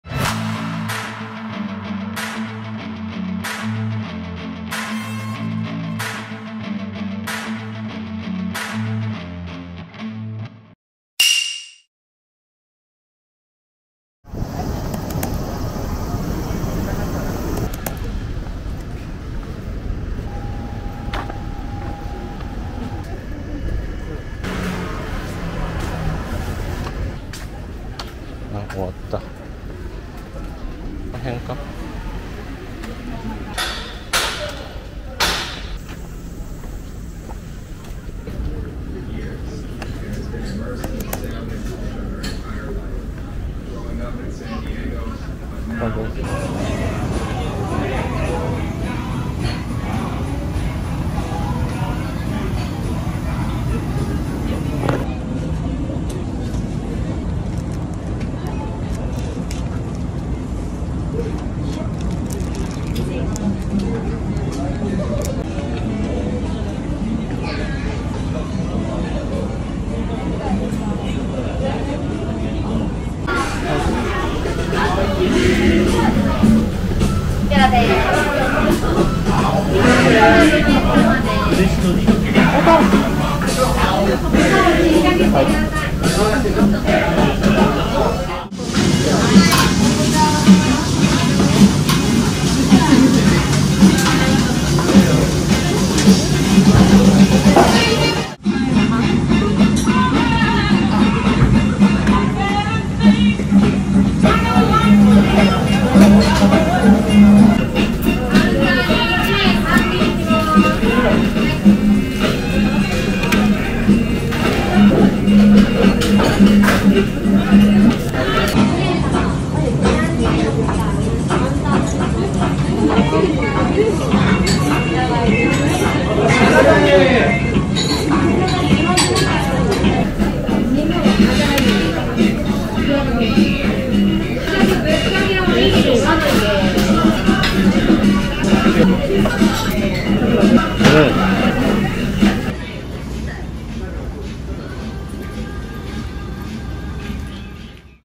Eat carbonara udon in Shibuya [Tokyo]
This time, I tried carbonara udon at the udon restaurant Mensan in the Shibuya and Harajuku areas.